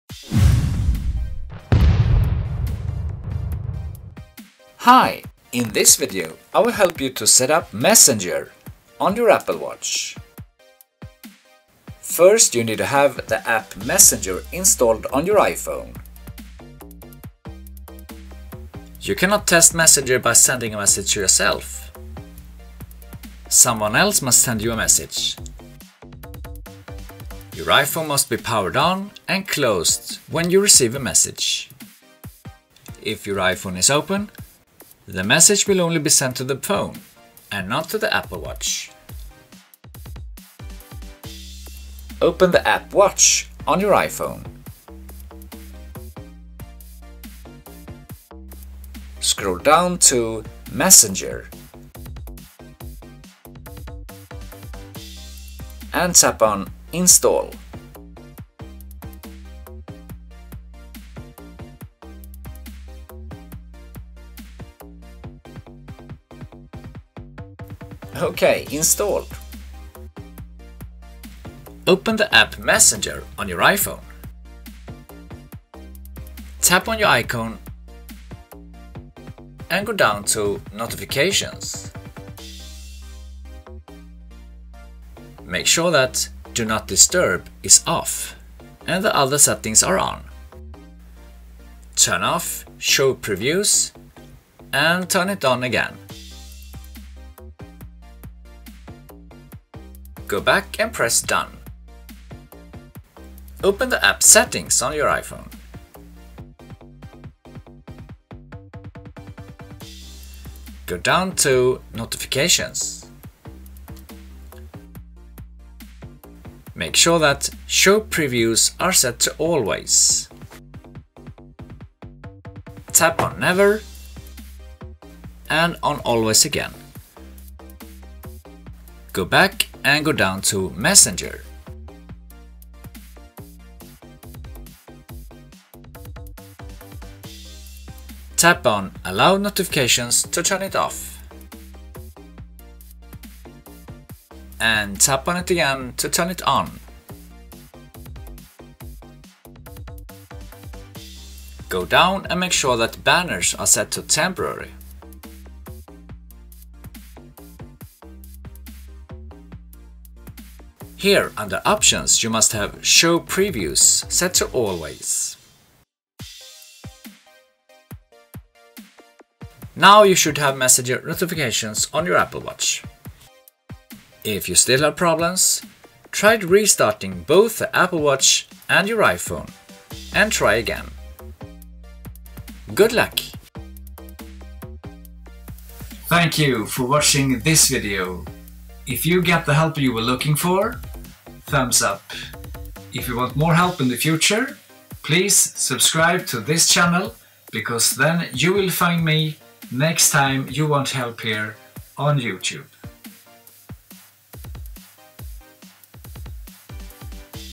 How to setup Messenger on Apple Watch
This video will help you to setup Messenger on your Apple Watch. Enable messenger notifications on Apple Watch. Setup Apple Watch facebook messenger.